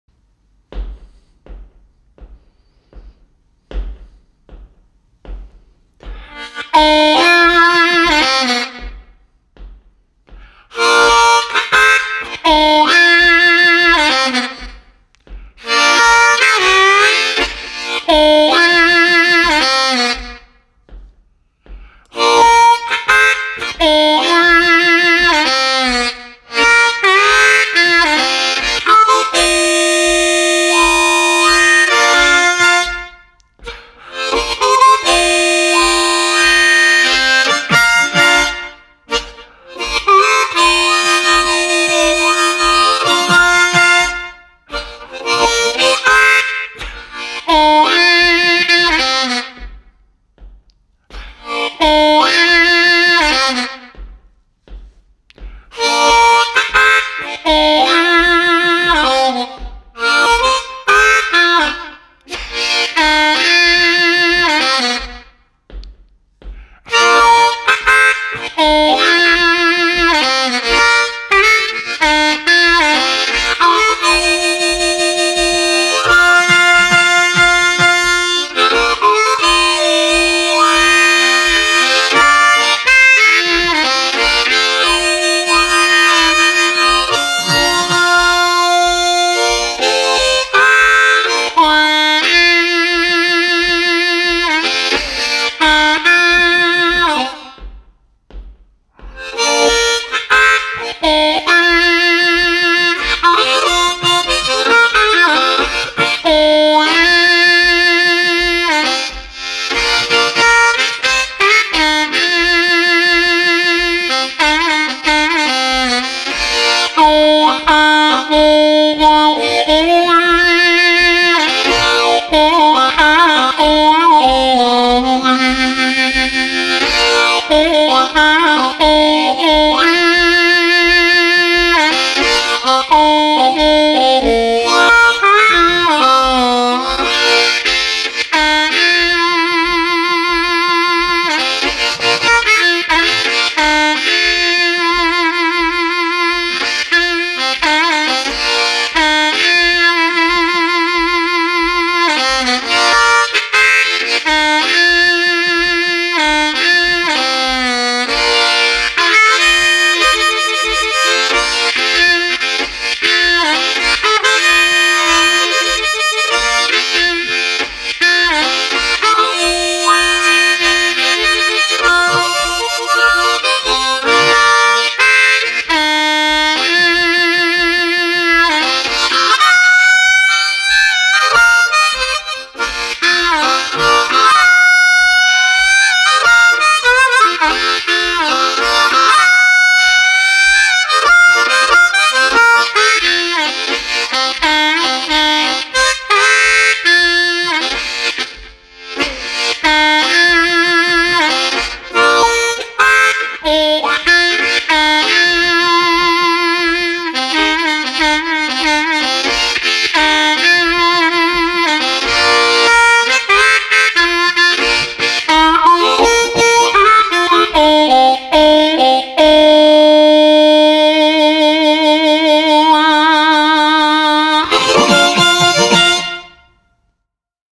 THEME FIFTY ONE BREAKS OR NO BREAKS?
In this video I show you how to start with a few licks that I combine with breaks to create more tension, but also to keep calm and overview of what I am playing.
After more than 1 minute I gradually start using fewer breaks to make the licks a bit more powerful.
I also use breaks to give listeners the opportunity to use their imagination and imagine for themselves what I might have played.
And also important, during the breaks you have time to breathe calmly 🙂
BenBoumanBlues